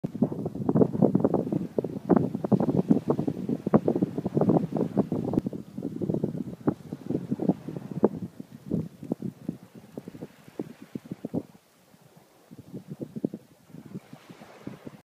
Ludington State Park. Lake Michigan. Feather in the wind.
A view of the wind blowing a feather caught in the sand on Lake Michigan. July 2024